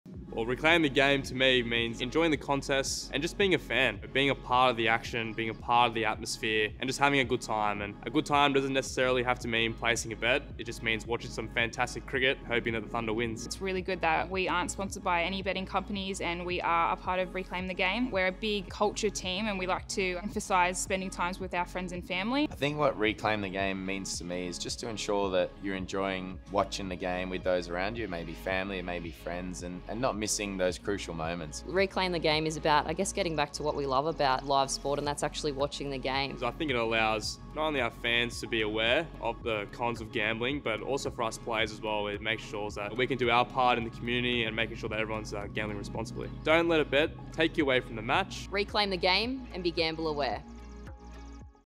Sydney Thunder - Why do we want fans to Reclaim the Game?
Hear from Thunder players including Jason Sangha, Sam Bates and Chris Green who share why they are so proud to see the Thunder partner with Reclaim the Game.

Don't let a bet take you away from the match.